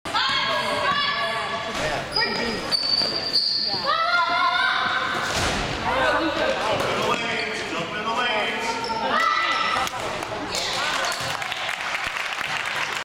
U12 T-Birds Basketball Game 10 - Blazers - Katie Finishes Spots
Sawyer brings the ball up court and the T-Birds begin their set by running "Spots".  After running through the rotation a few times Katie pops the ball up top to Sawyer and darts into the key.  Sawyer lobs a very nice pass that hits Katie on the run.  Katie lays it up and in for Two.